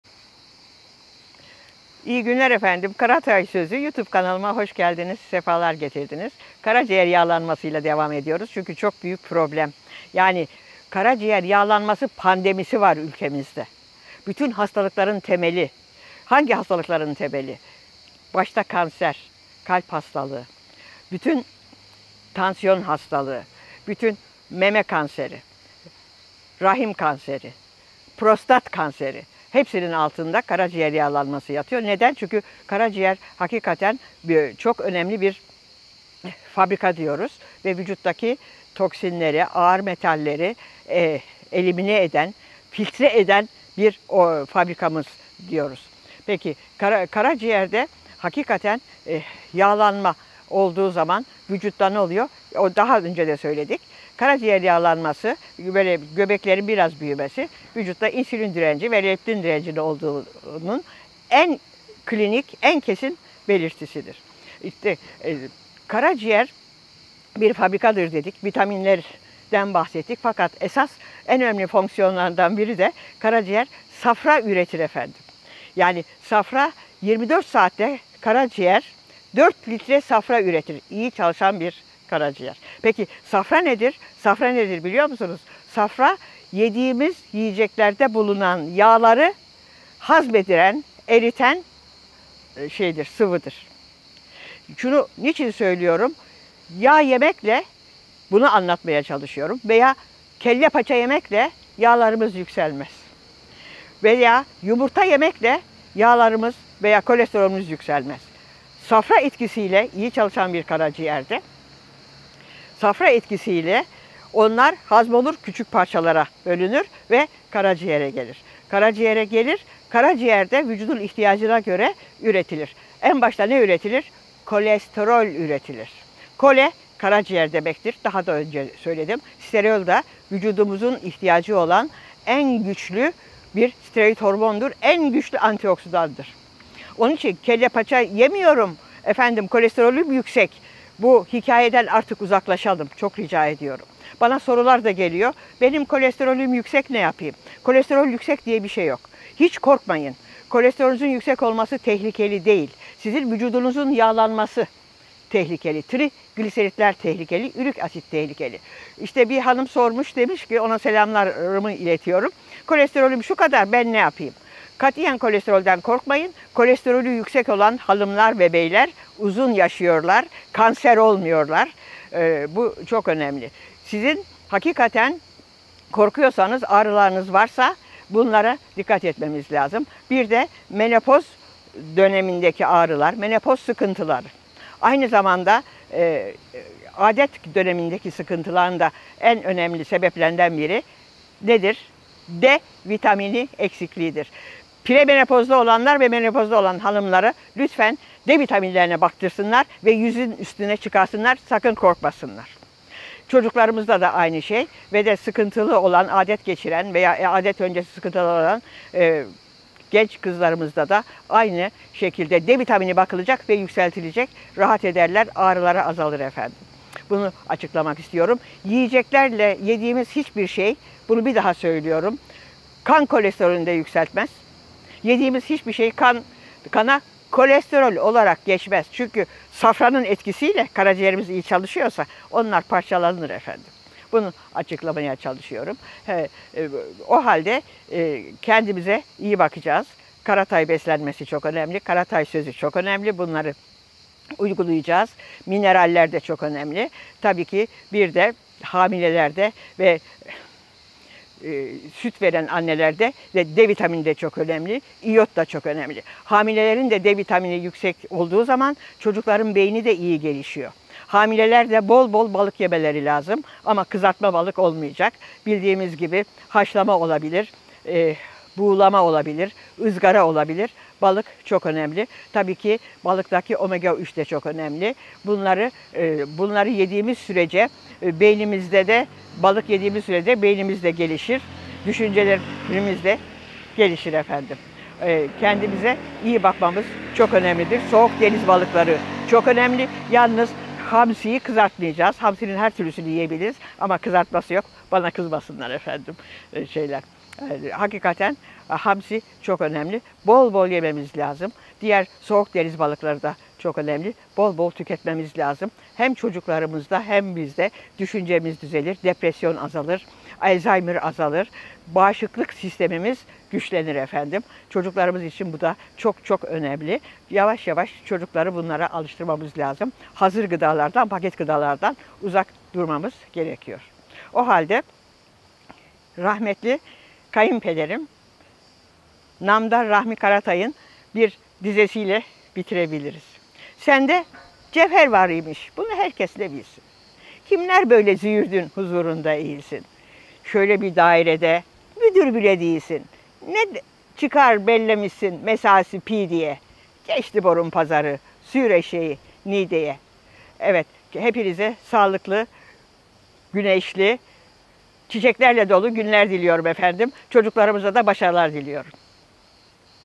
KARACİĞER NEDEN SAFRA SALGILAR? KARACİĞERDE SALGILANAN SAFRANIN GÖREVİ NE?
KARACİĞER NEDEN SAFRA SALGILAR?
KARACİĞERDEN SALGILANAN SAFRANIN GÖREVİ NE?

PROF. DR. CANAN KARATAY KARACİĞER VE SAFRA SAĞLIĞININ ÖNEMİNE DİKKAT ÇEKİYOR VE BESLENME İLE İLİŞKİSİNİ ANLATIYOR…

Videonun altına yorumlar kısmına genel sorularınızı yazabilirsiniz.